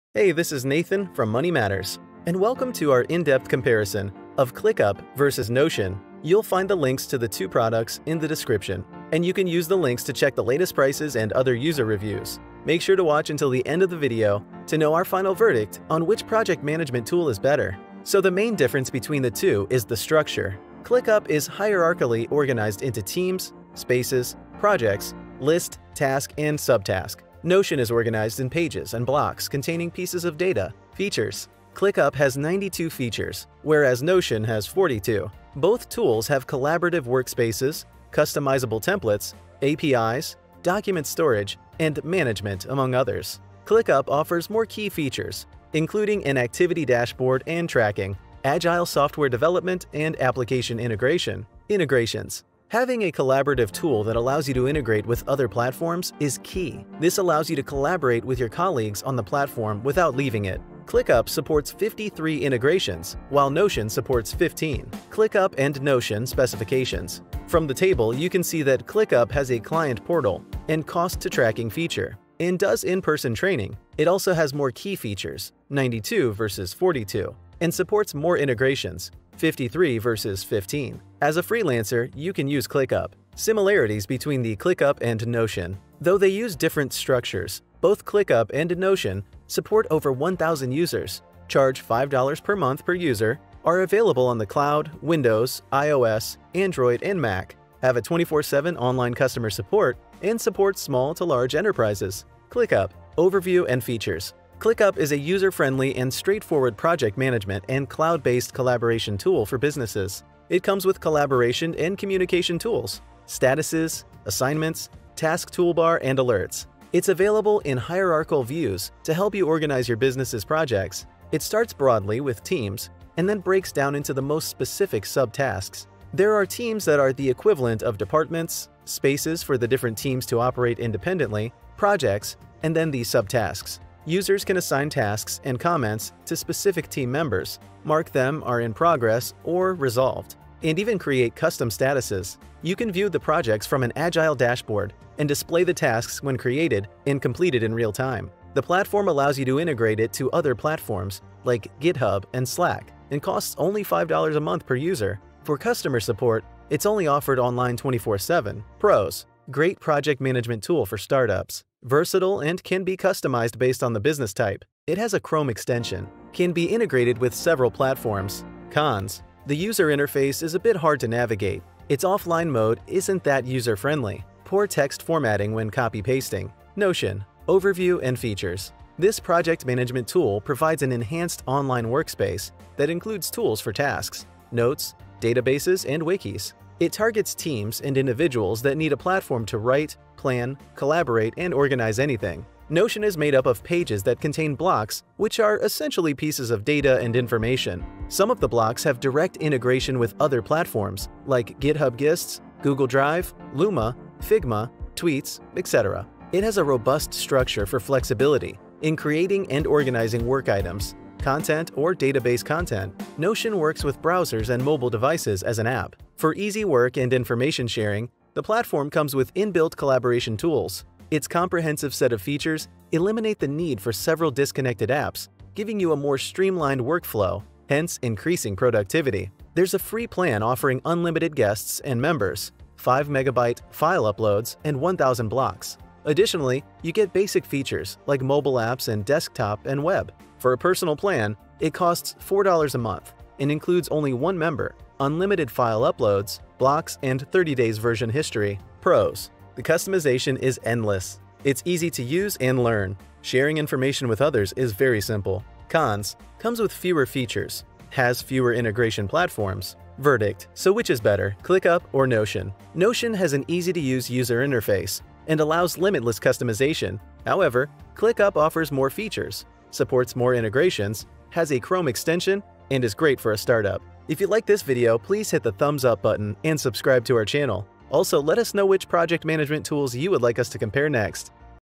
ClickUp vs Notion - Which is better?
Welcome to our in-depth comparison of Clickup vs Notion

So the main difference between the two is the Structure
ClickUp is hierarchically organized into Teams, Spaces, projects, List, Tasks, and sub-task. Notion is organized in Pages and blocks containing pieces of data.

which one is better ClickUp or notion?